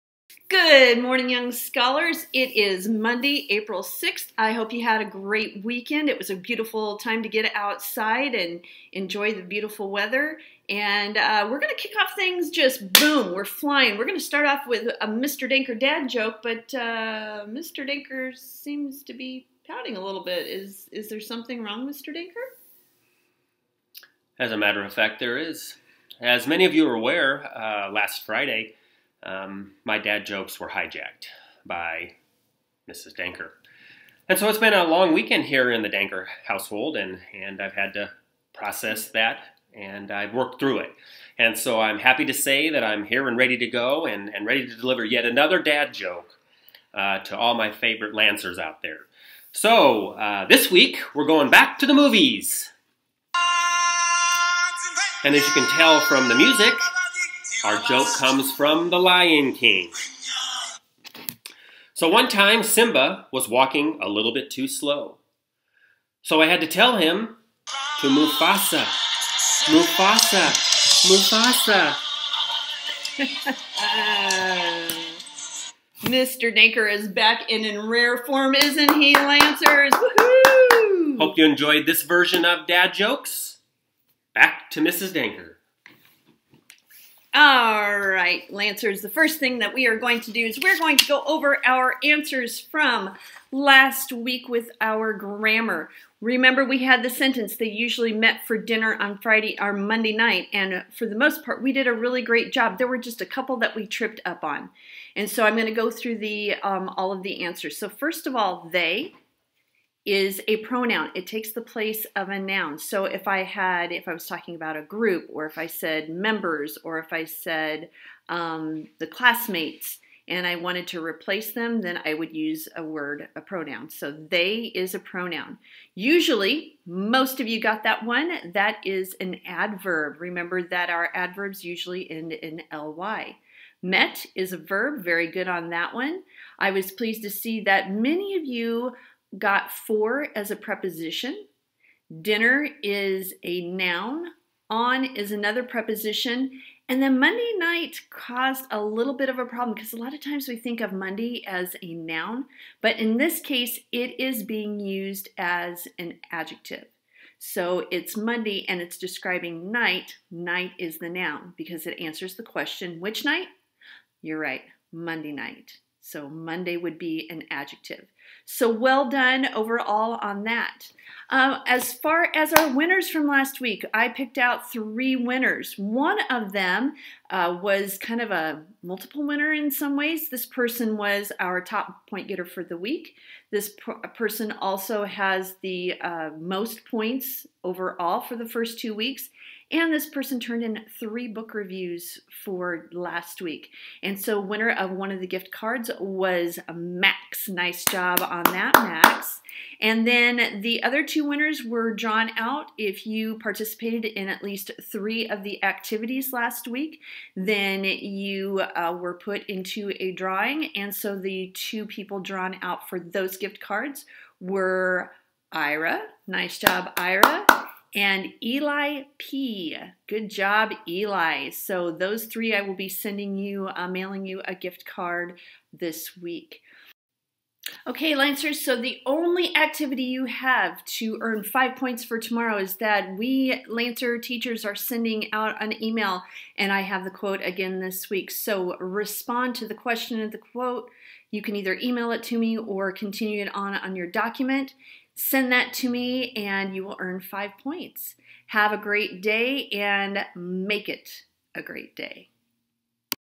Mr. Denker Returns!
Mr. Denker is back with a dad joke, we go over our grammar sentence, and you are given your challenge for the day. ***Note: The challenge for the day is associated with the Lancer email, but it will not come out until 6:00 tonight (the time got changed from this morning but after I had already completed the video).